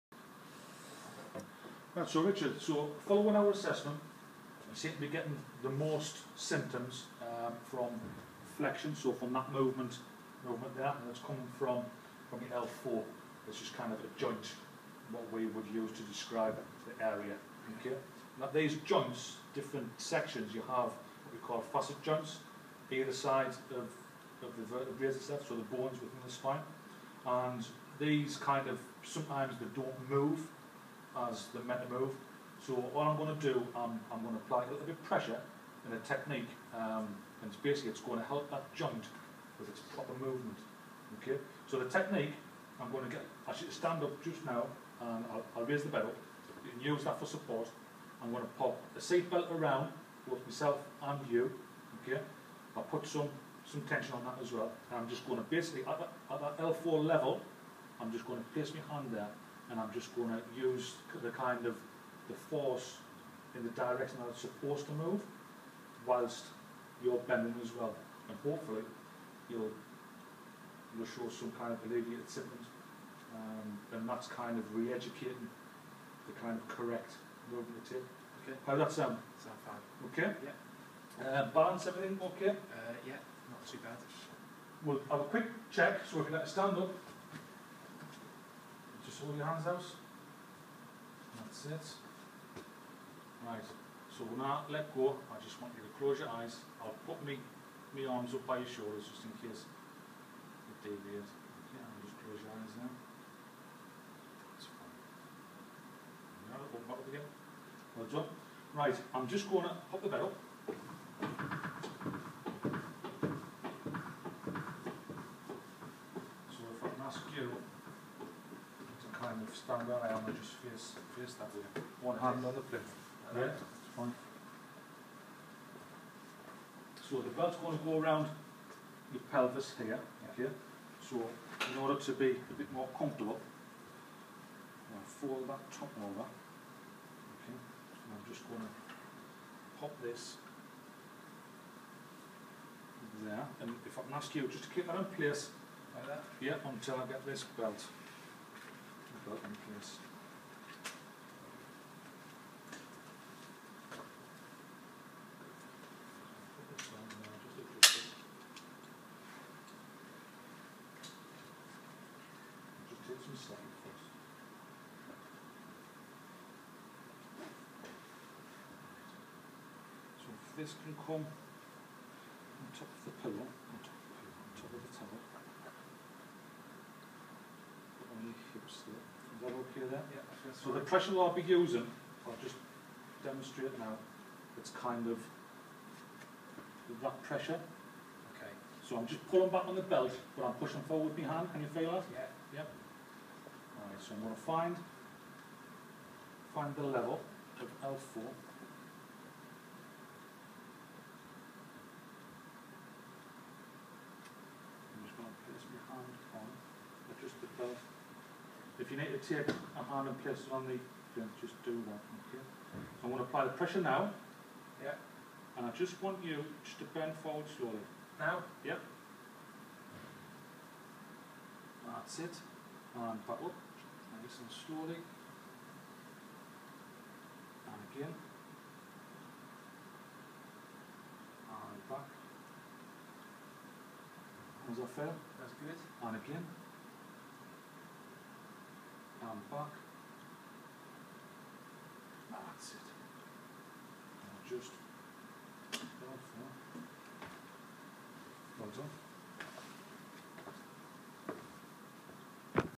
Sustained Natural Apophyseal Glide (SNAG) to lumbar spine
Sustained Natural Apophyseal Glide (SNAG) to lumbar spine to gain correct pain free movement of the lumbar spine.